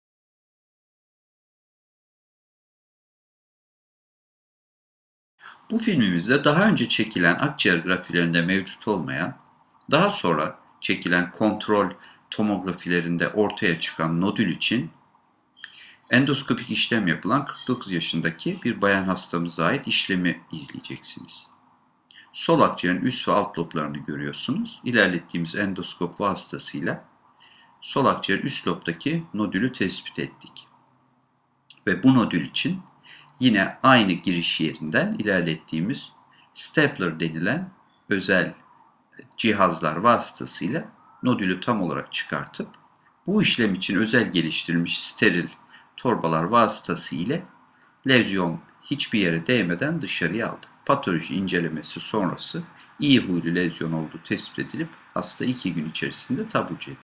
ACİĞER NODÜLü ÇIKARTILMASI OPERASYONU
Akciğerlerimizde zaman zaman küçük yuvarlak lezyonlar ortaya çıkabilir. Nodül adı verilen bu lezyonlar çoğu zaman iyi huyludur. Bununla birlikte şüpheli nodüller yakından izlenir ve eğer düzelme olmaz veya büyüme olursa cerrahi olarak çıkartılmaları gerekebilir.
Bu videomuzda bir akciğer nodülün kapalı bir operasyon ile nasıl çıkartıldığı görülmektedir.